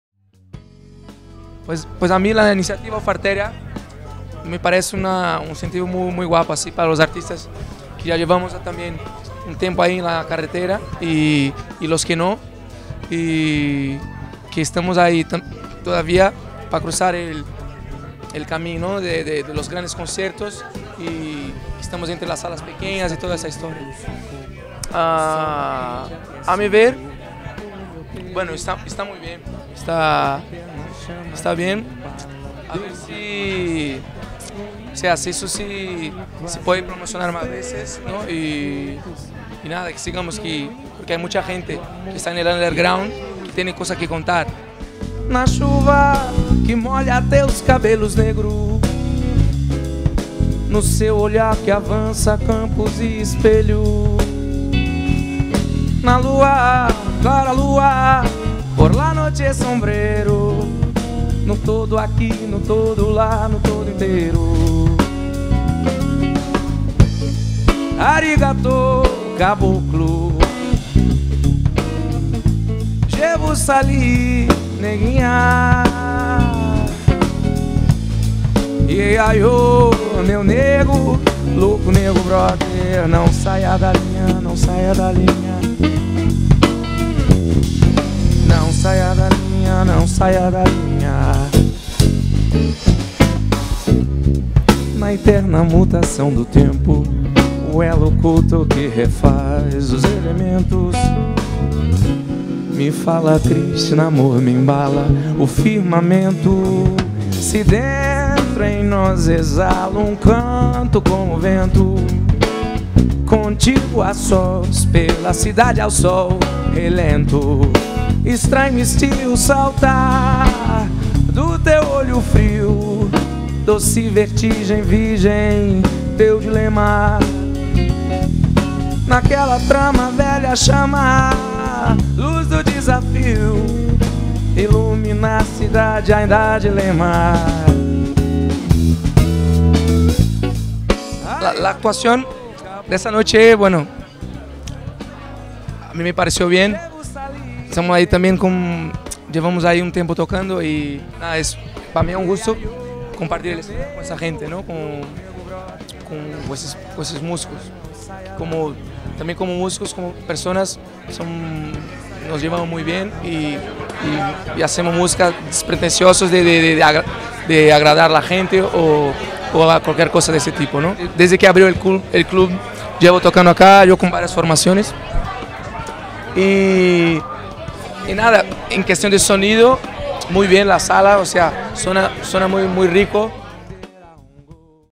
Joao Oliveira.
Ésta es una propuesta musical de temas cantados con elementos instrumentales nada convencionales, su música nace de sus orígenes, de la tradición brasileña cantada con la cálida expresión que la caracteriza y acompañada con ritmos intensos, funk, rock y colorismo latino.